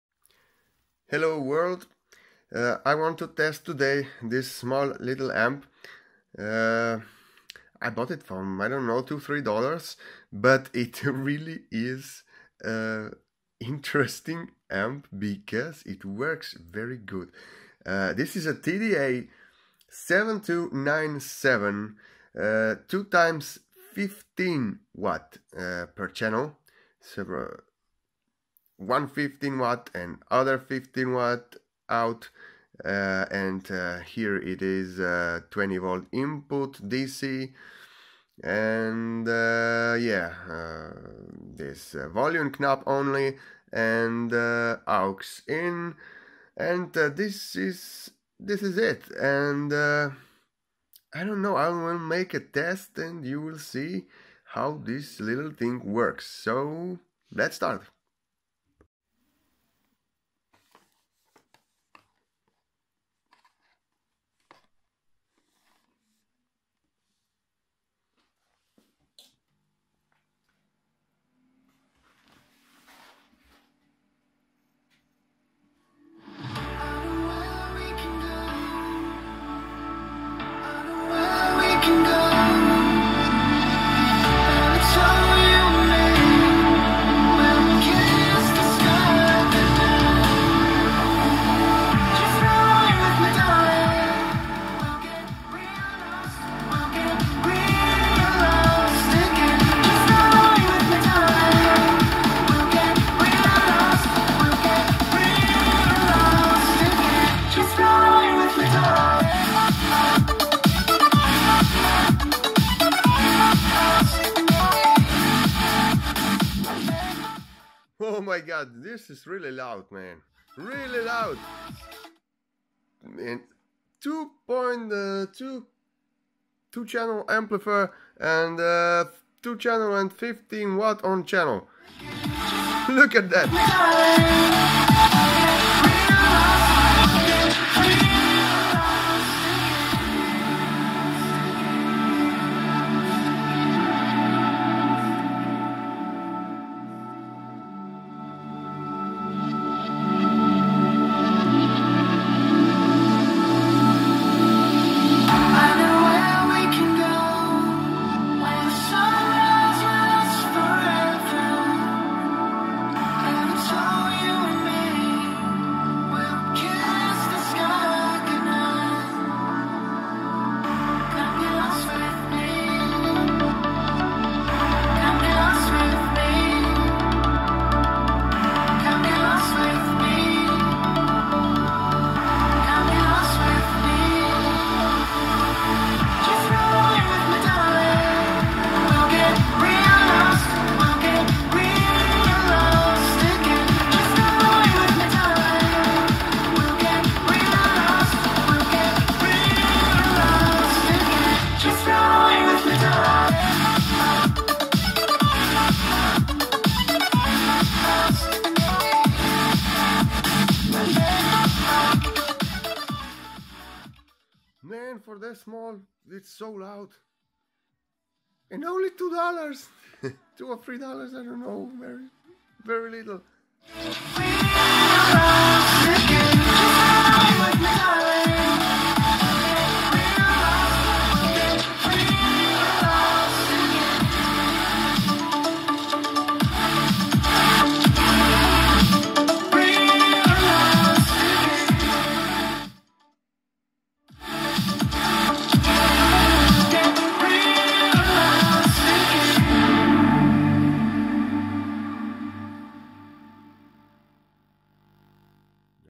TDA 7297 2x15W - Mini amplifier sound test
Small circut board but it looks that 15W is enough to get speakers loud.. and eaven enough for small party..